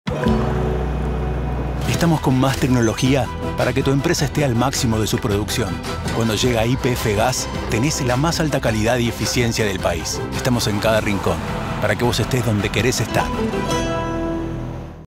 YPF GAS
En YPF GAS estamos en cada rincón de la Argentina, para que vos estés donde querés estar. Abastecemos con energía segura a empresas, industrias y campos de todo el país. Brindamos más tecnología, rendimiento y la más alta calidad. Tenemos la mayor cobertura del país y el mejor servicio para vos, conocelo en ypfgas.com.ar  🔎